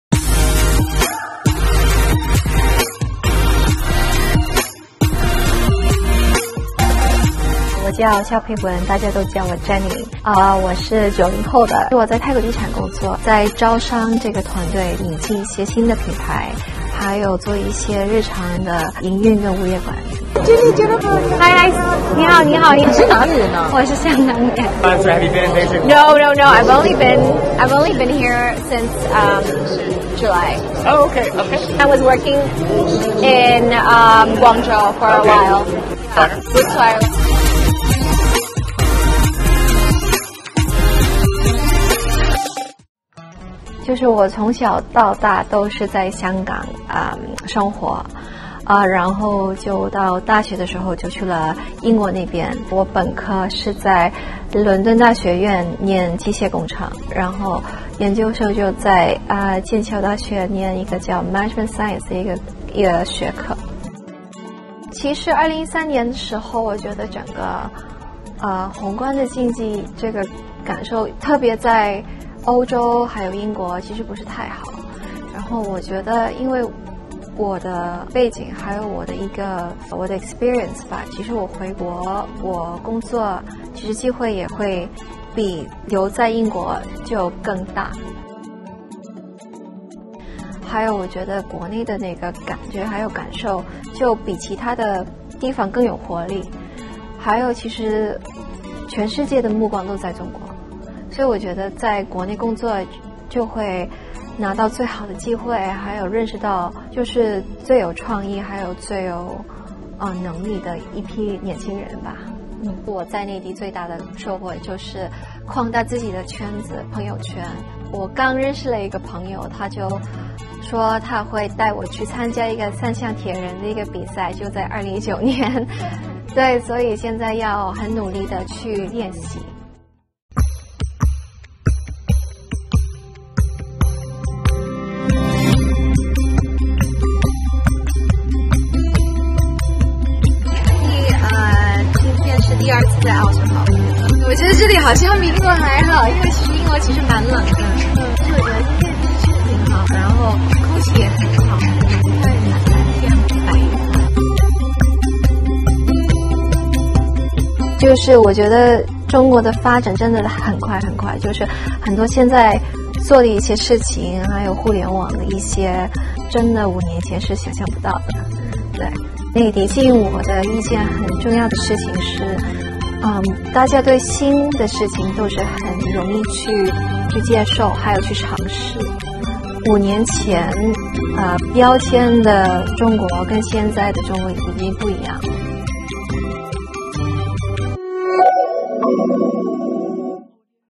全世界的目光都在内地！剑桥女学霸爱上内地的N个理由
来自香港的萧沛雯，是英国剑桥大学毕业的90后女学霸，在国外的经历让她发觉“全世界的目光都在内地”，她选择回到内地工作生活，在这里她也认识了一批很有能力的年轻人，发现当今的内地已经完全不一样了. 妥妥地靠实力圈粉，根本停不下来！